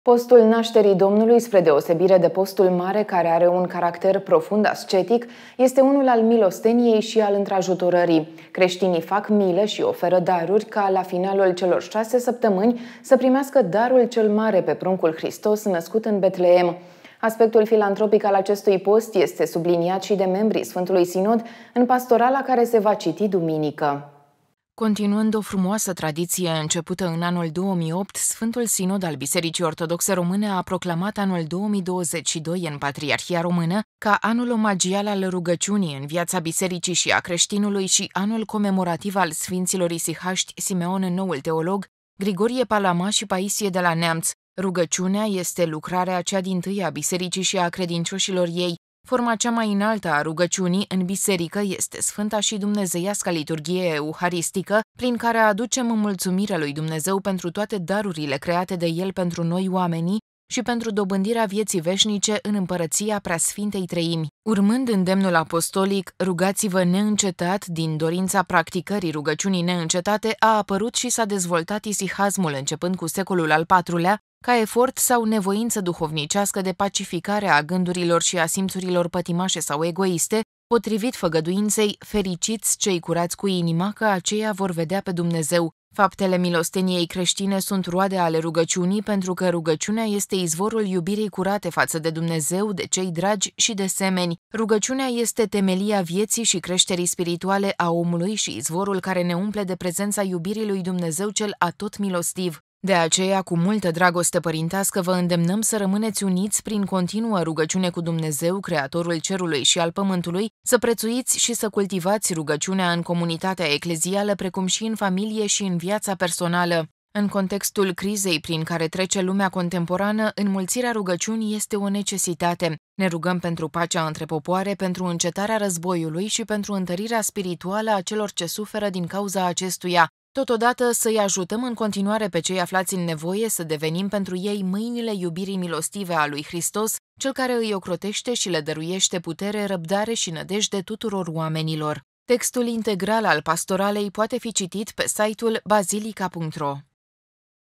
Pastorala Sfântului Sinod la prima Duminică din Postul Nașterii Domnului
Postul Nașterii Domnului, spre deosebire de Postul Mare care are un caracter profund ascetic, este unul al milostieniei și al întrajutorării. Creștinii fac milă și oferă daruri, ca la finalul celor 6 săptămâni să primească Darul cel Mare, pe Pruncul Hristos, născut în Betleem. Aspectul filantropic al acestui post este subliniat și de membri Sfântului Sinod, în Pastorala care se va citi duminică.